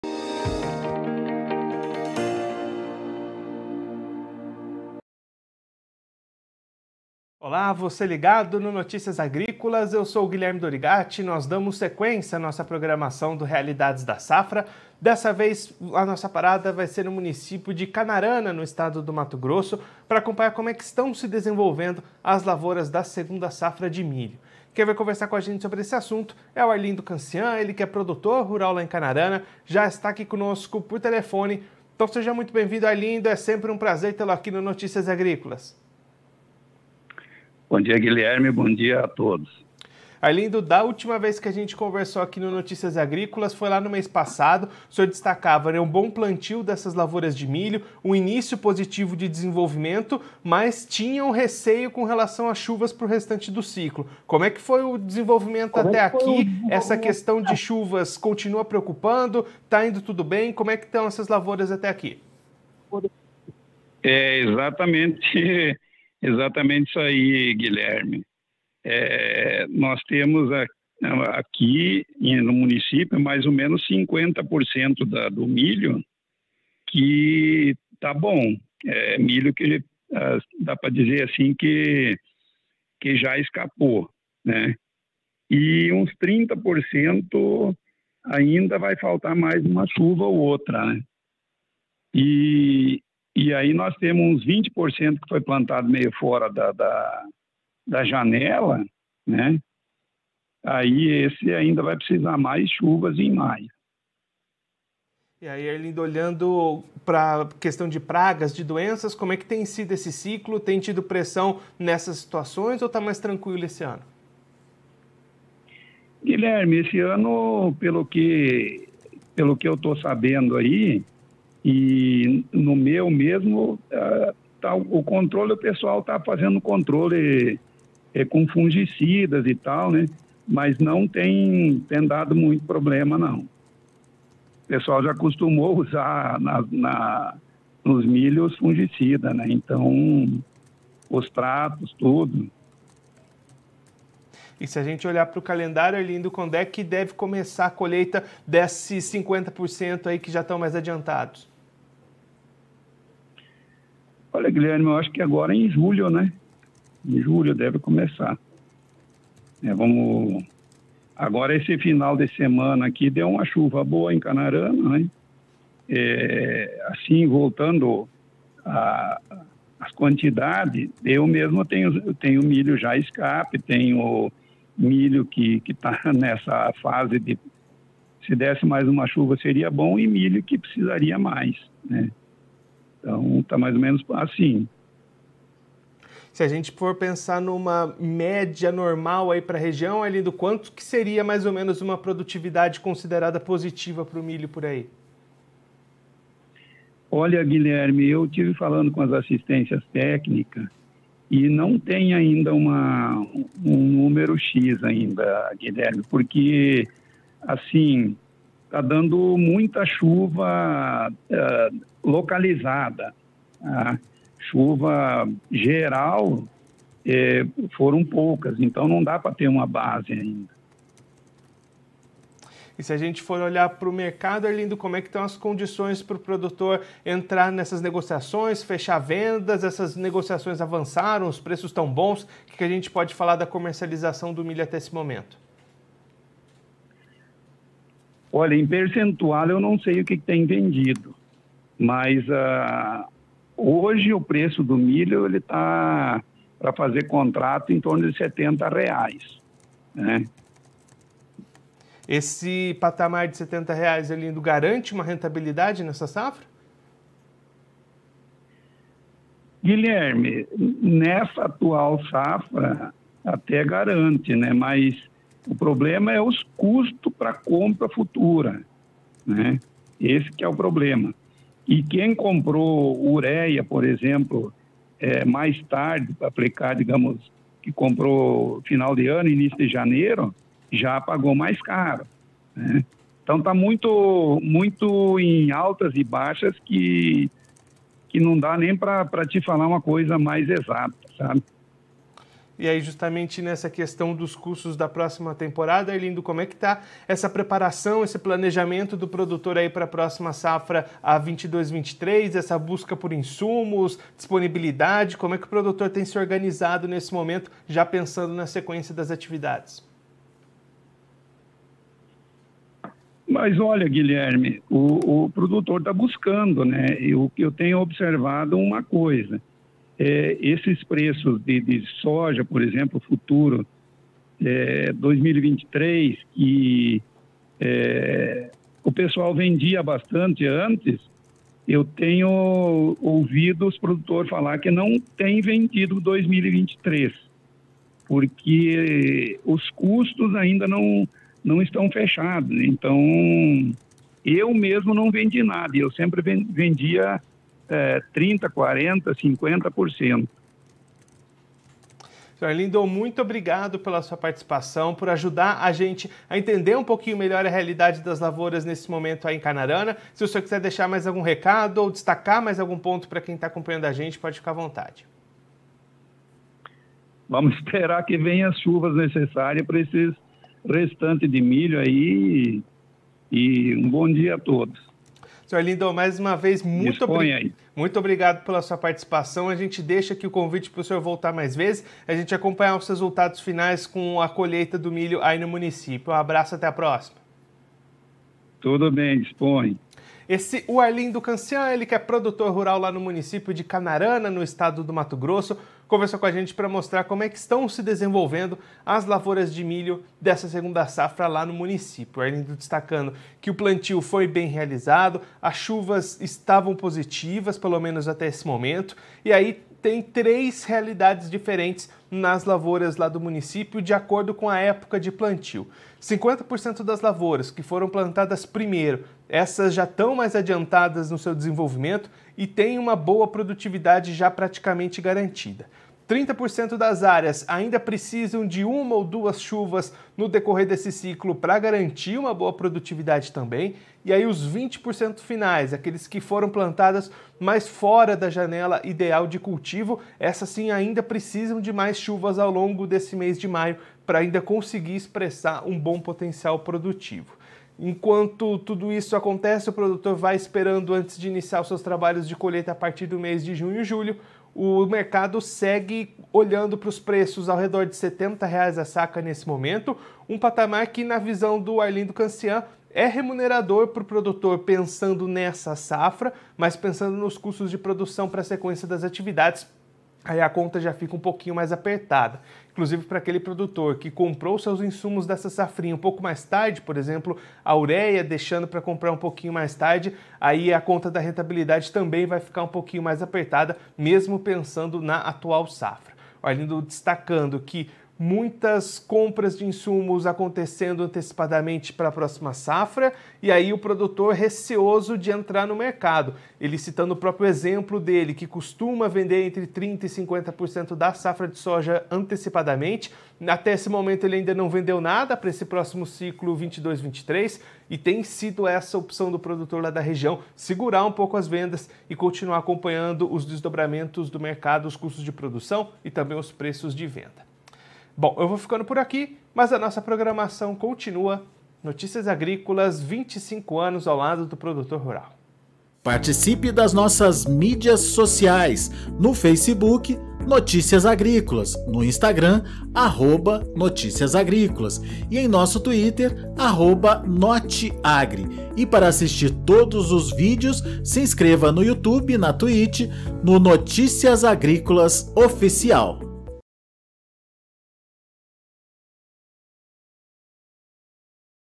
Pelo menos 50% das lavouras de milho em Canarana/MT já tem boa produtividade garantida
Da metade restante, 30% precisam de mais uma ou duas chuvas e 20% vão precisar de mais precipitações ao longo de maio